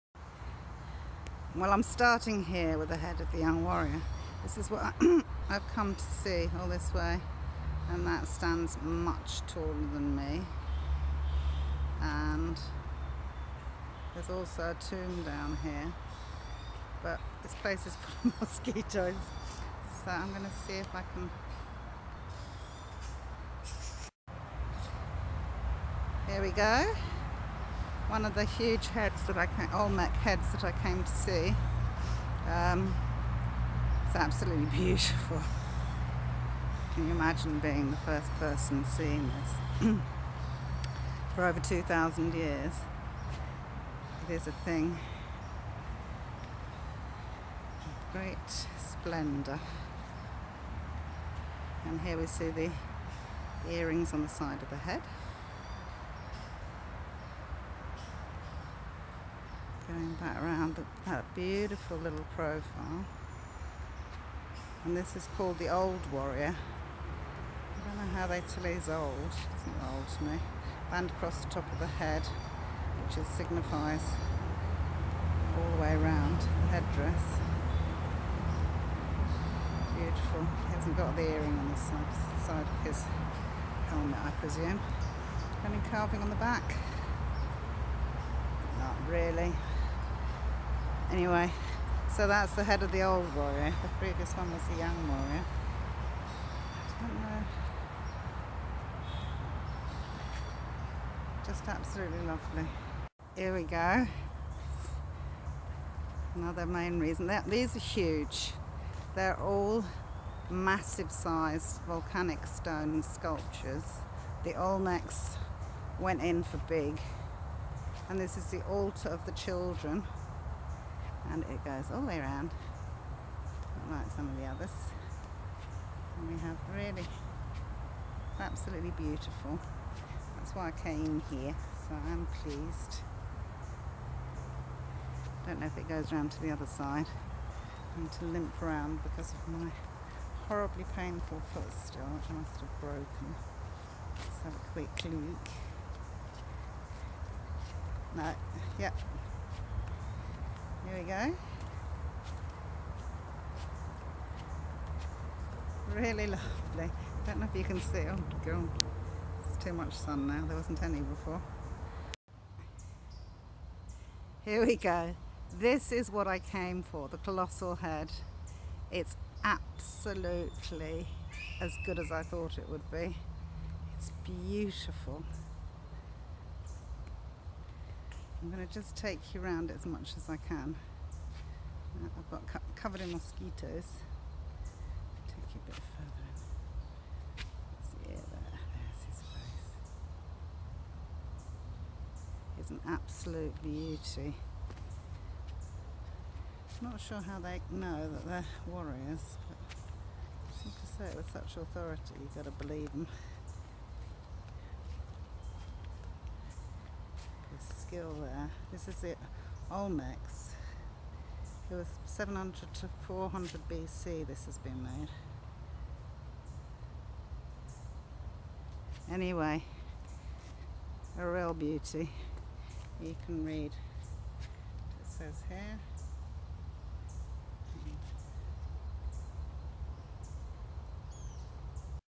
OLMEC GIANT HEAD SCULPTURES
In Villahermosa Tabasco Mexico, the Olmecs thrived from 1200 to 350 BC and left a great legacy of colossal sculptures from basalt a kind of volcanic rock. These are some of them.